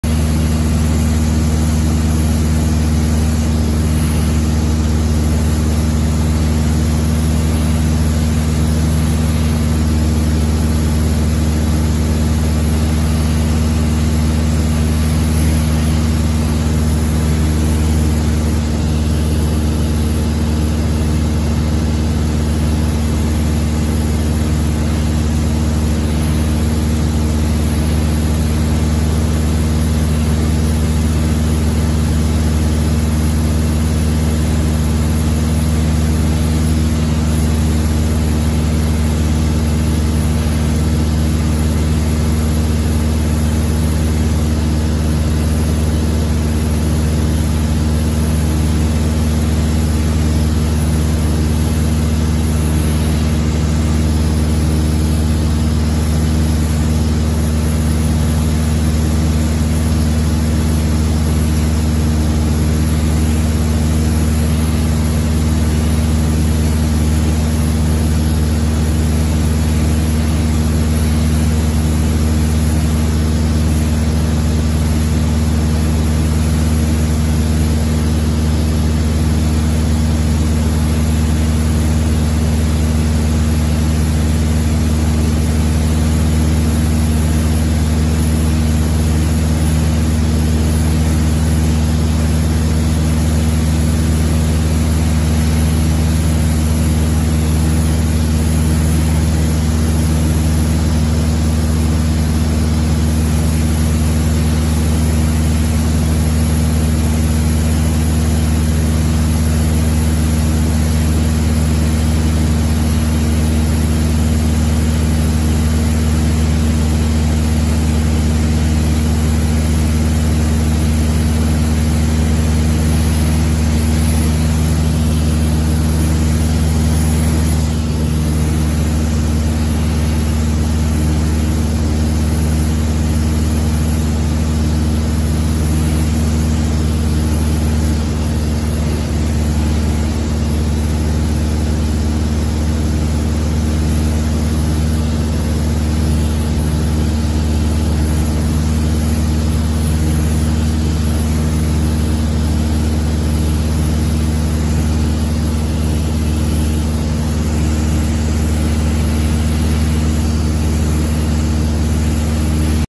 Last of the ships heading down bound January 15, 2022.
Last of the ships heading down bound today January 15, 2022. Locks close today. I see a barge full of Algoma Steel coils in the parade. One of the ice breakers was working around it.

That’s got to be one cold place to work this time of the year!